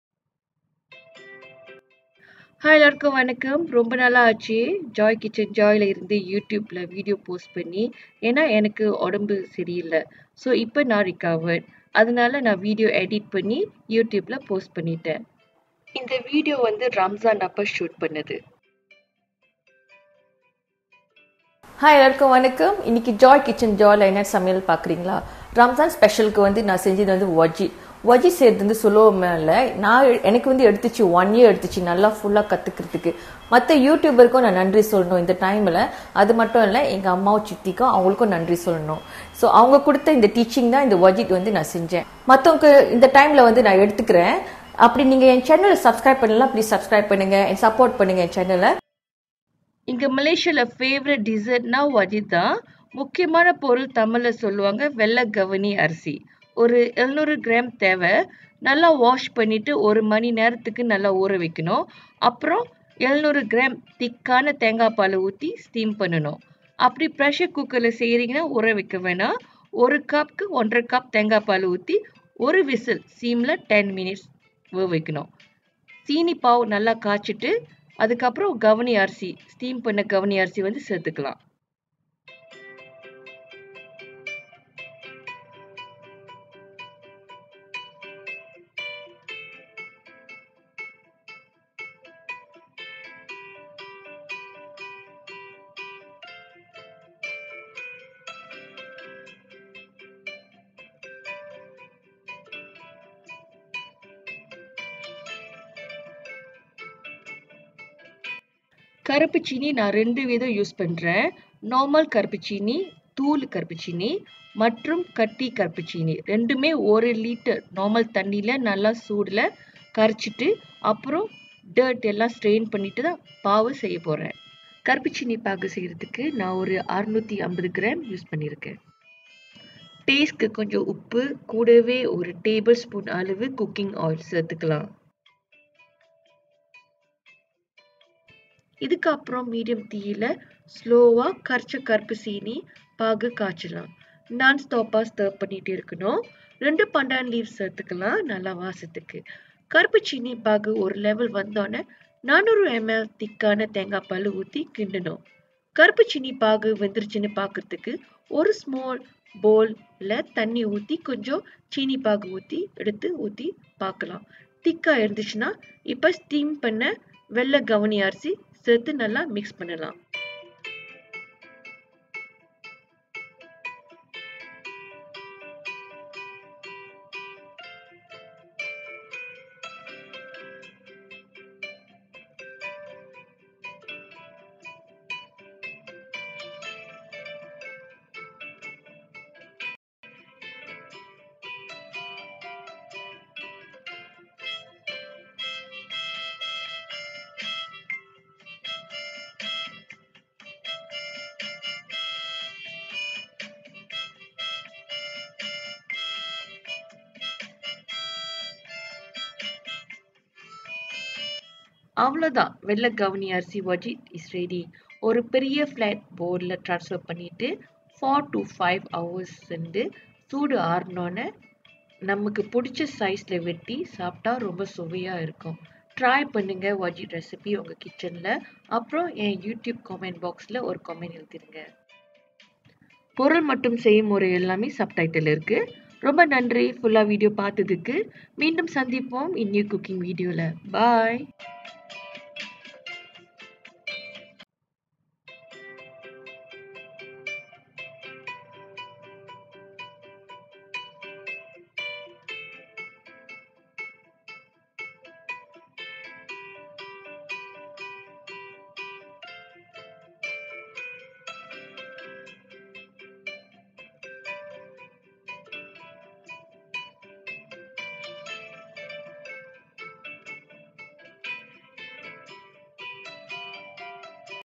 Wajik Pulut with Tips | Vela Gavani Arisi Barfi - Tamil Audio (After Long Time Posting Video)
Dessert WAJIK Recipe

English
Glutinous Rice Steam Ingredients: -
700g Glutinous Rice / Gavani Rice (Tamil)
700ml Thick Coconut Milk
1 Tsp Salt
2 Pandan Leave

Sugar Caramel Ingredients: -
250g Stick Brown Sugar (Gula Melaka)
300g Brown Sugar (Black Colour) mil)
400ml Thick Coconut Milk
1/2 Tbsp Salt
1 Tbsp Cooking Oil
2 Pandan Leave
1 Ltr Water

Bahasa
Bahan Bahan Beras Pulut untuk dikukus: -
700g Beras Pulut / Glutinous Rice (English)
700ml Santan Kelapa Pekat
1 Sd Kecil Garam
2 Daun Pandan

Bahan-Bahan Karamel Gula Pekat: -
250g Gula Merah Batu (Ketul)
300g Serbuk Gula Merah
400ml Santan Kelapa
1/2 Sd besar Garam
1 Sd besar Minyak Masak
2 Daun Pandan
1 Ltr Air

Thank you for your continous support.

Facebook / Instagram ID : Joy Kitchen Joy